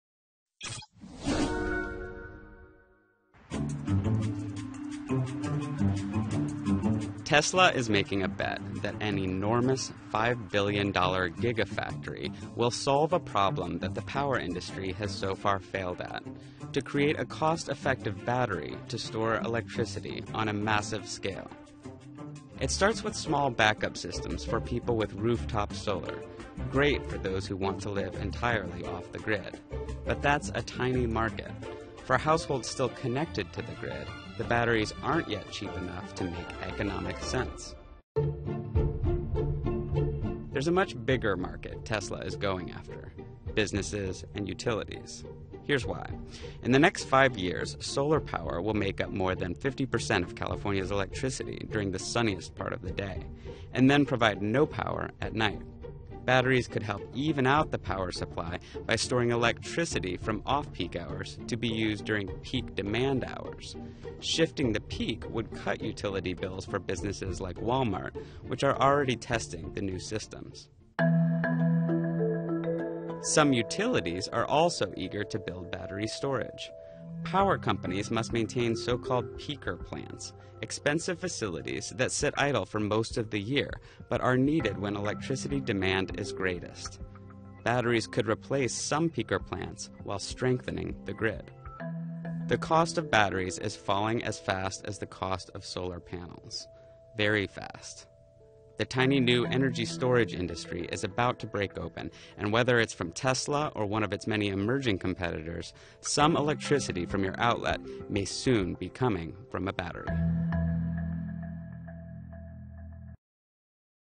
Tesla Invests in Pollution-Free Energy Production
Tesla Motors Inc. invests $5 billion in a gigafactory of batteries to store electricity for homes, businesses and utilities. Chief Executive Officer Elon Musk says that a greener power grid furthers the company’s mission to provide pollution-free energy.
“Our goal here is to fundamentally change the way the world uses energy,” he said at an event Thursday at the company’s design studio in Hawthorne, California. “We’re talking at the terawatt scale. The goal is complete transformation of the entire energy
infrastructure of the world.”